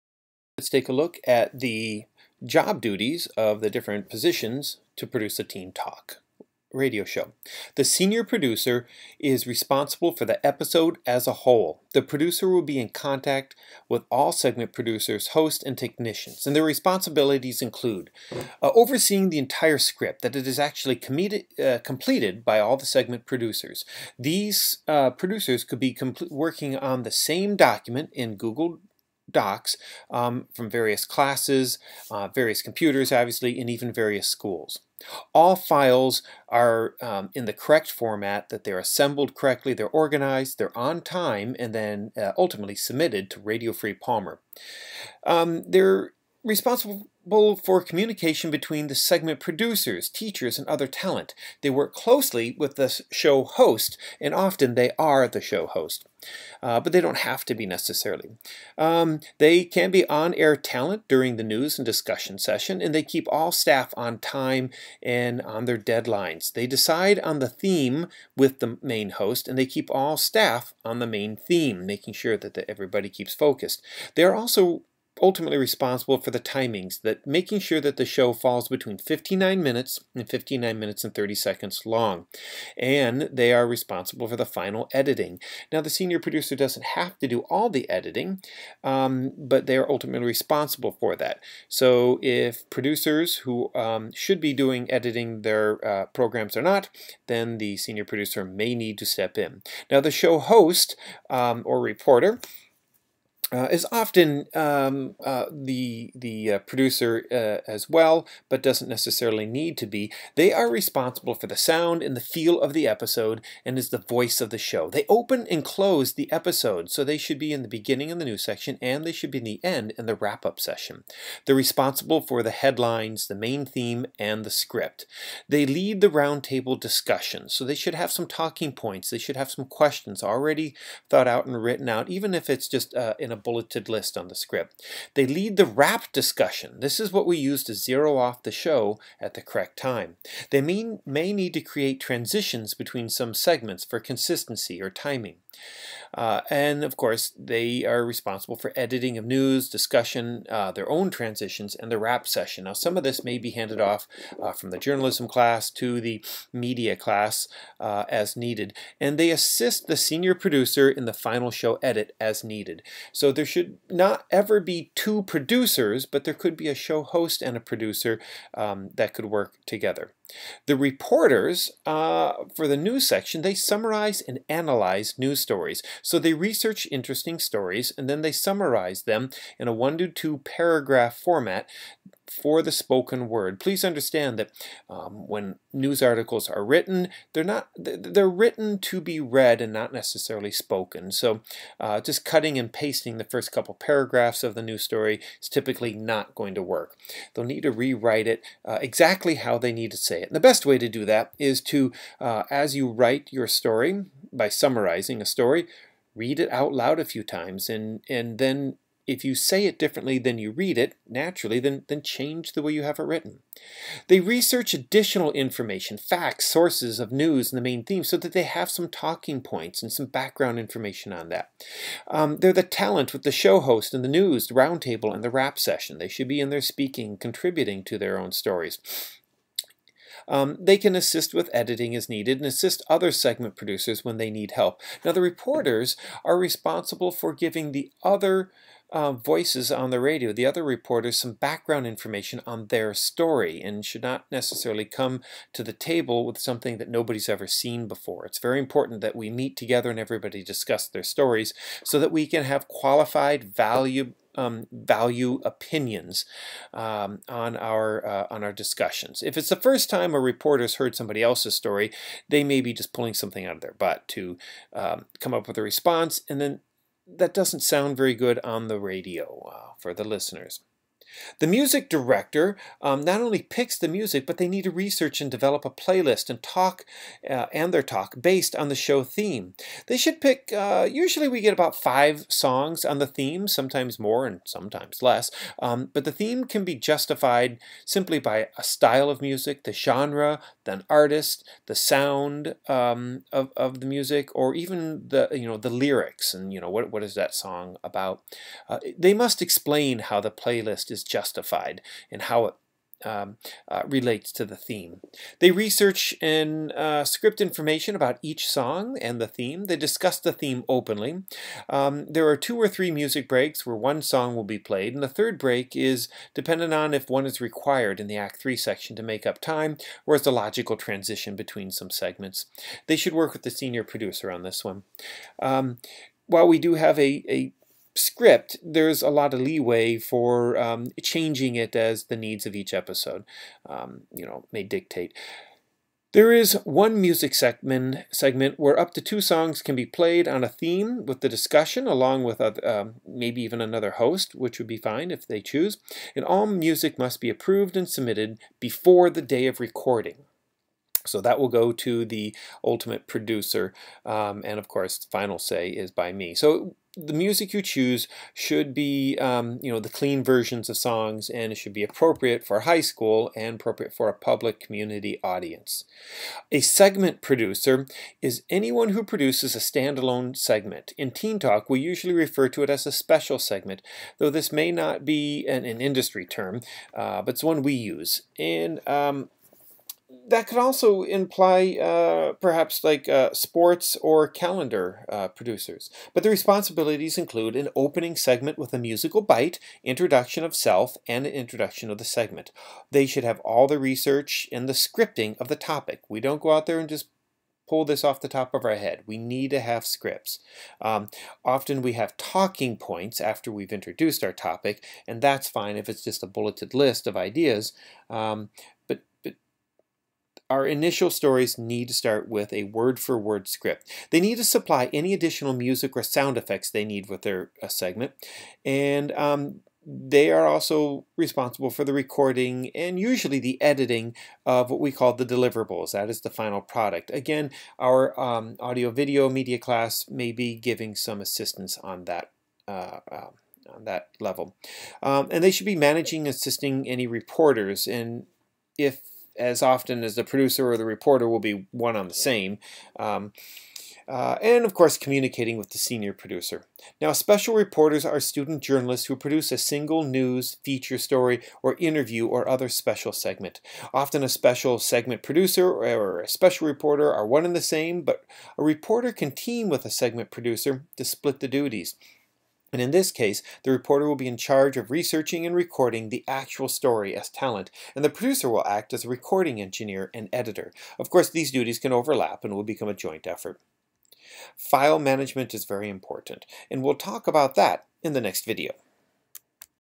Teen Talk Job Descriptions
Job duties for all of the positions in the Teen Talk radio show.